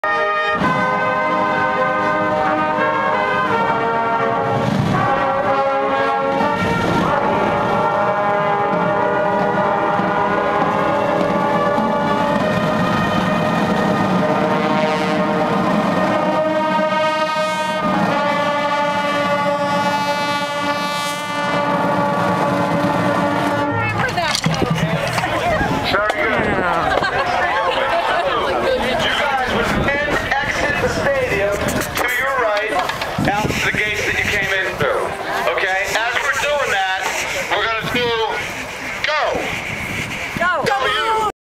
All the Marching alumni T-Bones!
A run down of the 2010 UMMB alumni Trombones, from left to right, including two rows worth of folks for the first ~20 yards!